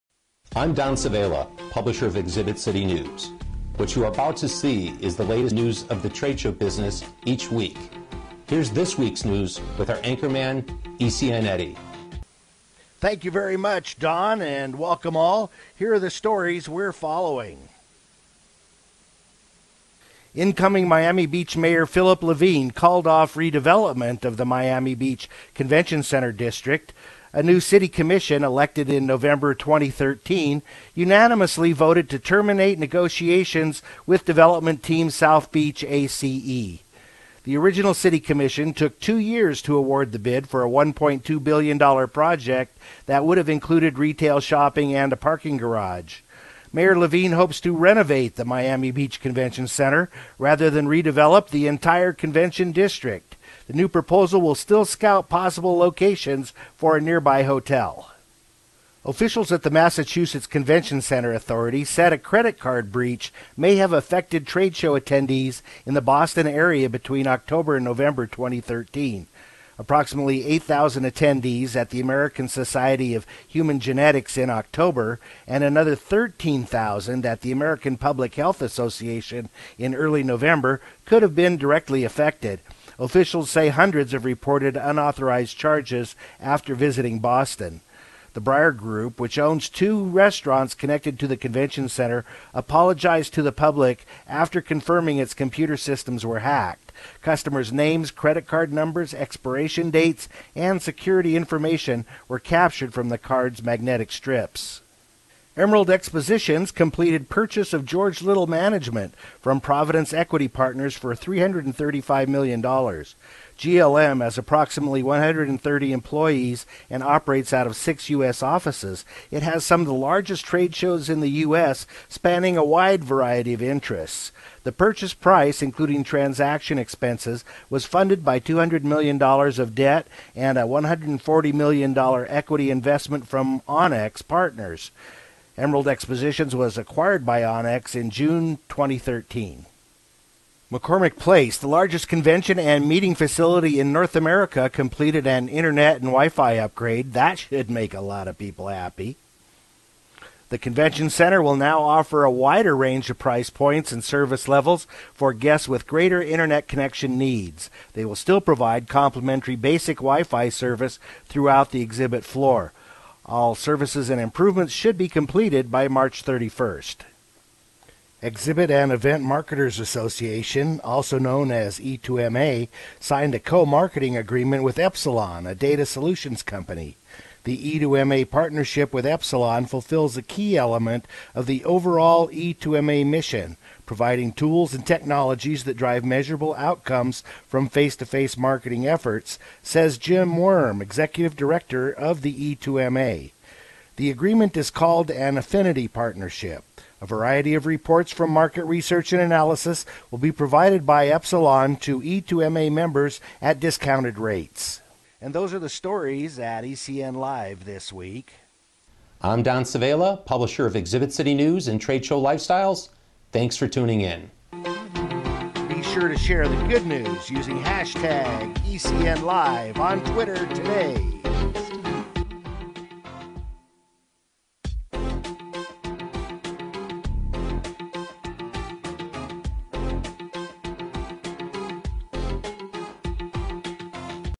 ECN Live! Week of Jan. 18-24, 2014
Featured stories:
 - Miami Beach revokes $1B convention center project
 - Boston credit card breach reflects security failure
 - Emerald Expositions acquires GLM
 - McCormick Place upgrades Internet, Wi-Fi
 - E2MA welcomes Affinity Partner Program

ECN Live! is a weekly newscast by Exhibit City News, the nation's only tradeshow industry news magazine.

ECN Eddie is a creation of CHOPS & Assoc. Live Animation, a 3D interactive avatar who speaks in real time. © CHOPS & Assoc. Live Animation